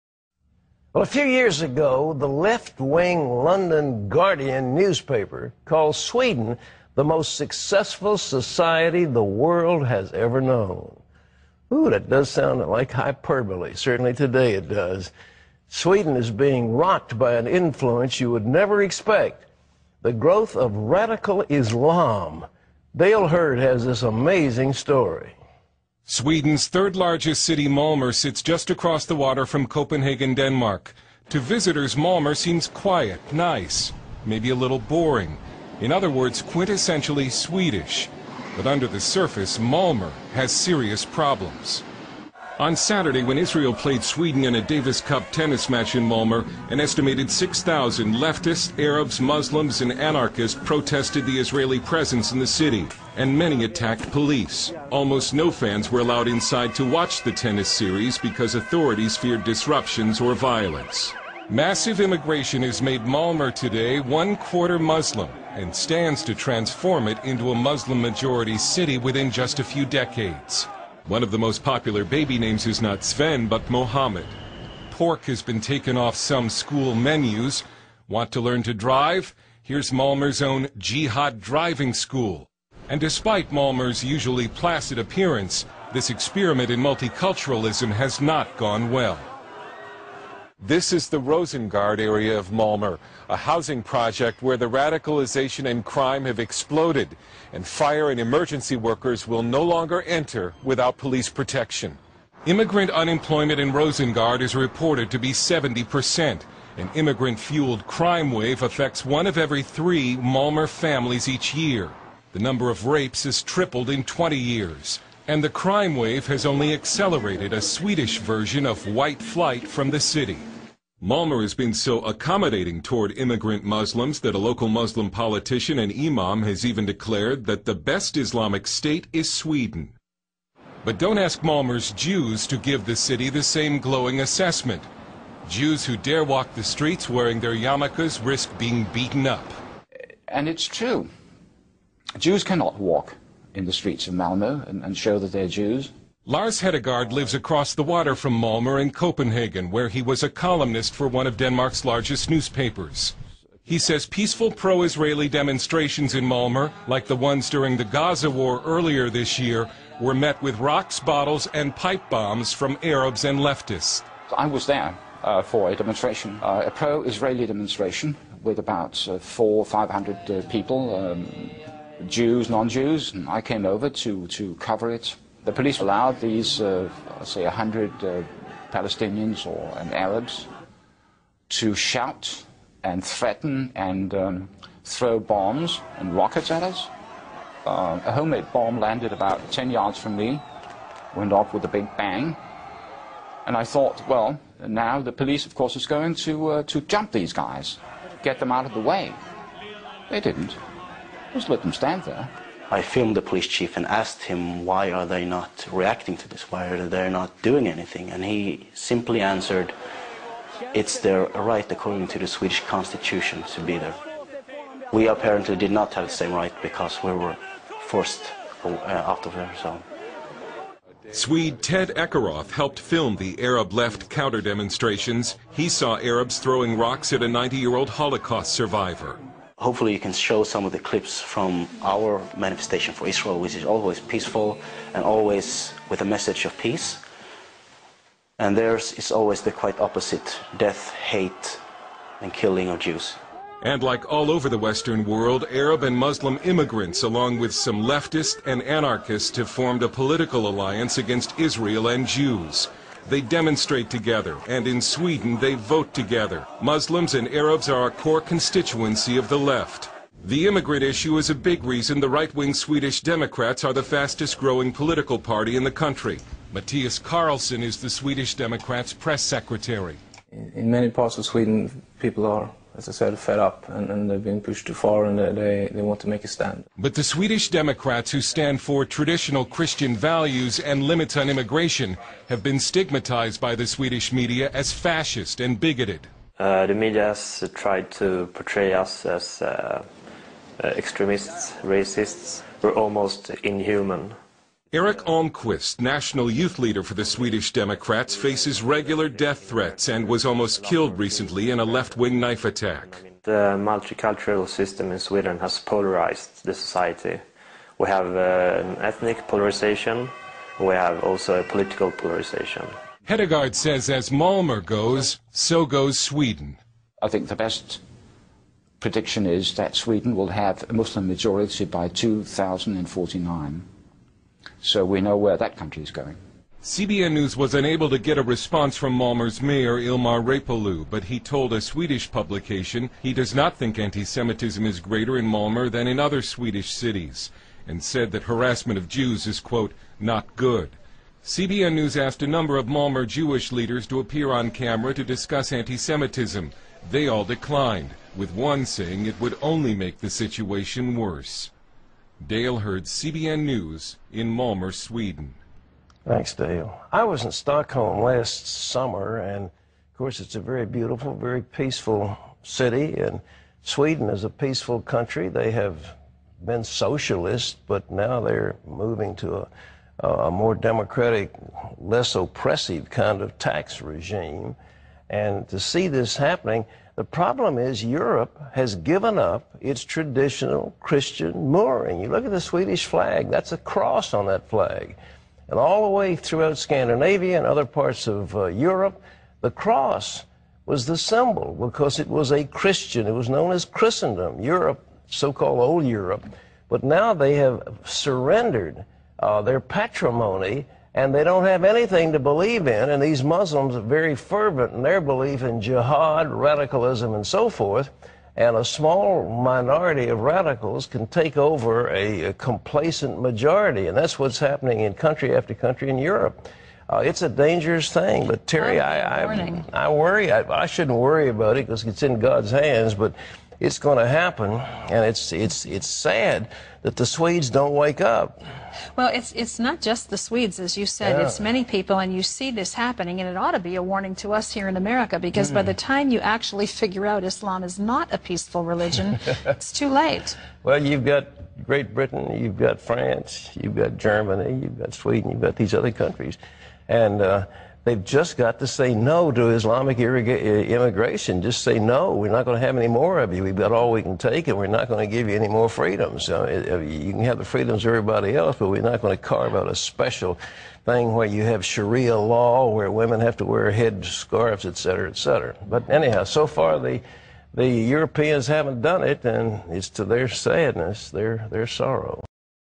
The Problem in Sweden WITH THE RELIGION OF PEACE "ISLAM"
The Problem of Islam in Sweden Is Islam A RELIGION OF PEACE or VIOLENCE?
Don't ask Malmö's Jews to give the city the same glowing assessment. Jews who dare walk the streets wearing their yarmulkes risk being beaten up.
"I was there for demonstration; a pro-Israeli demonstration with about 400 or 500 people," Hedegaard told CBN News. "Jews and non-Jews, and I came over to cover it.
The police allowed, I'd say a hundred Palestinians or Arabs to shout and threaten and throw bombs and rockets at us. A homemade bomb landed about ten yards from me, and went off with a big bang. And now of course, I thought the police were going to jump these guys, get them out of the way. They didn't. They just let them stand there."

Political Alliance Against Israel
And like all over the Western world, some on the Left, along with Arabs and Muslims and anarchists, have formed a political alliance against Israel and Jews. They demonstrate together, and in Sweden, they vote together. Muslims are a core constituency of the Left.

Swede Ted Ekeroth helped film the Arab-Left counter-demonstrations. He saw Arabs throwing rocks at a 90-year-old holocaust survivor.

"I filmed the police chief and asked him why are they not reacting to this," Ekeroth said. "Why are they not doing anything? And he simply answered, 'It's their right according to the Swedish constitution.' We apparently did not have the same right, because we were forced out of there. Our manifestation for Israel is always peaceful, and theirs is always the quite opposite -- Death, hate and killing of Jews. They come and they shout different slogans," he continued. "It can be everything from Arabic slogans inciting killing of Jews to in Swedish and Danish, 'Kill the Jews.'